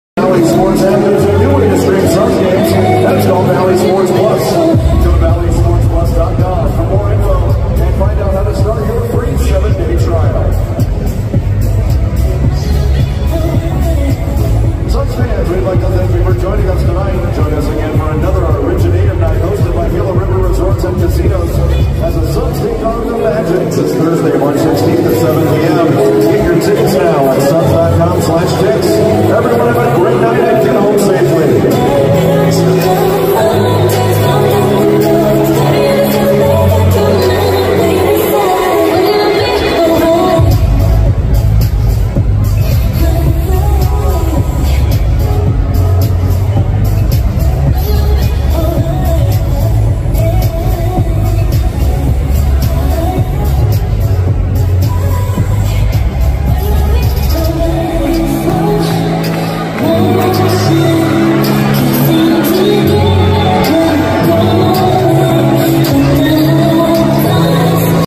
Jae Crowder hugging his former suns teammates and coaches after the game
and unique moments of the NBA players!
OWNERS!